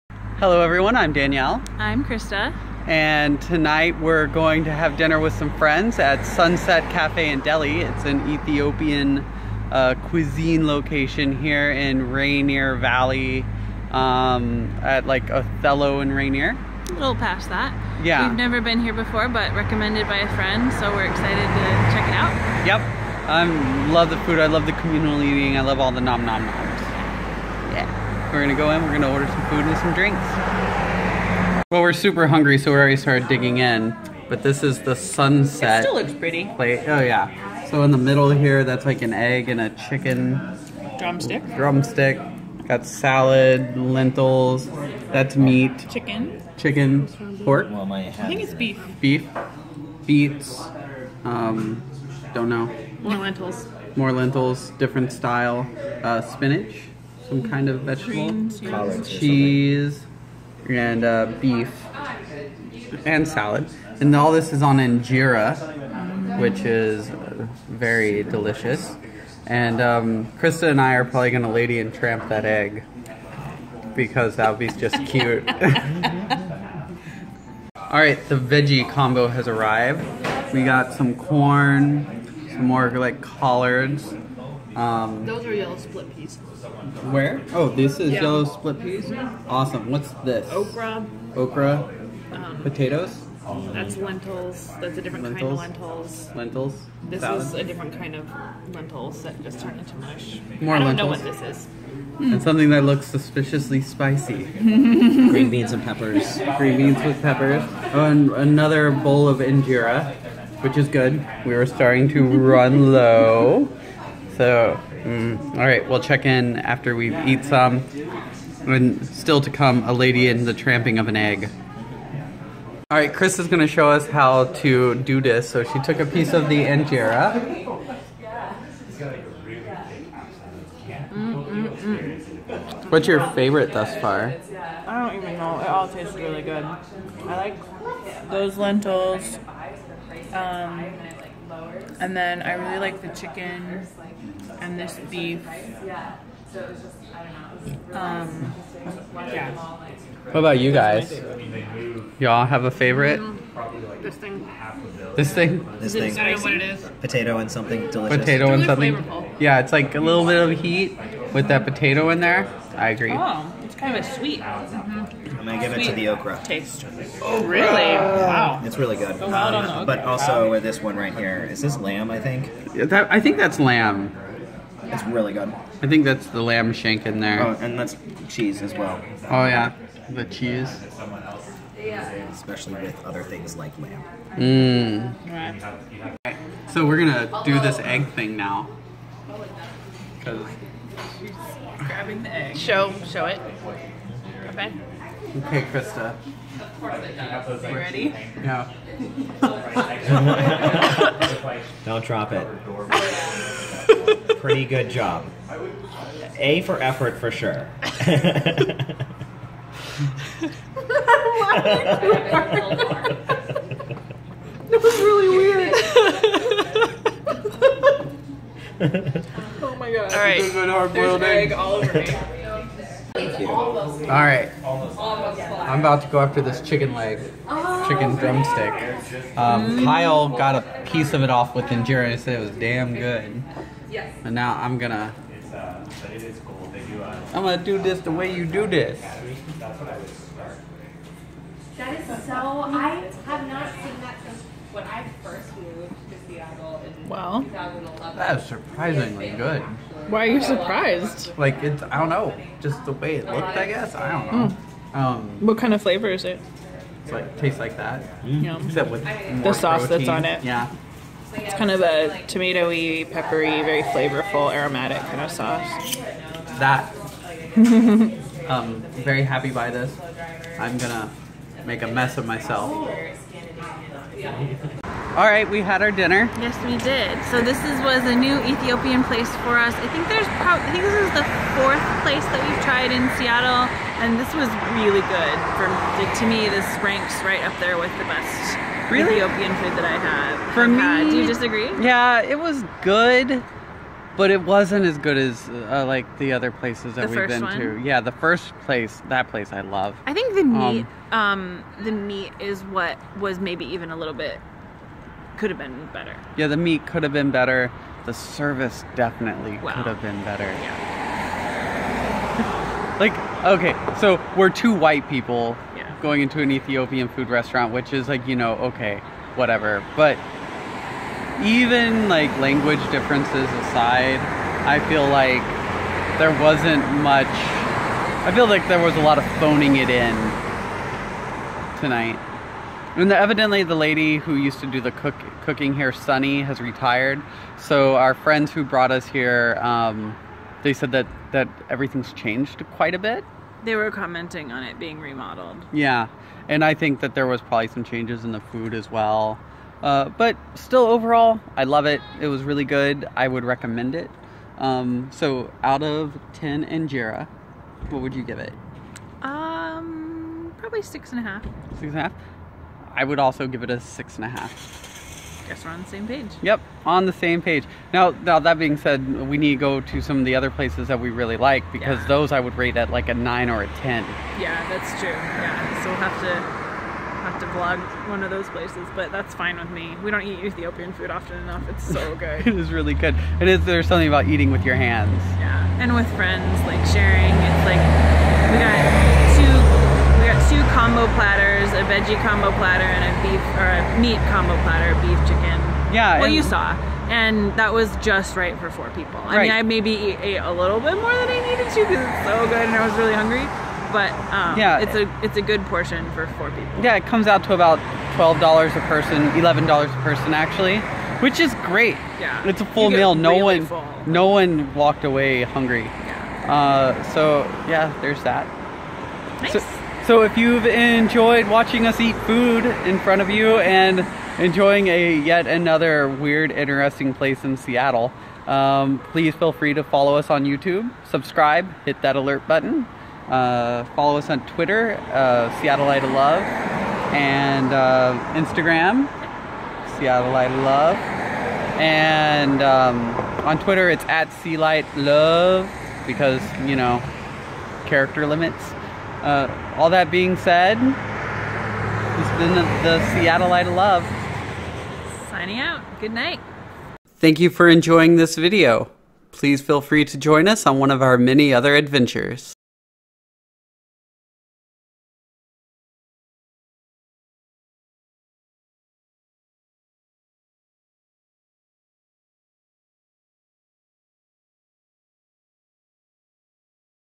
The Seattleite of Love @ Sunset Cafe & Deli
We have some nom nom noms at Sunset Cafe & Deli. Ethiopian Cuisine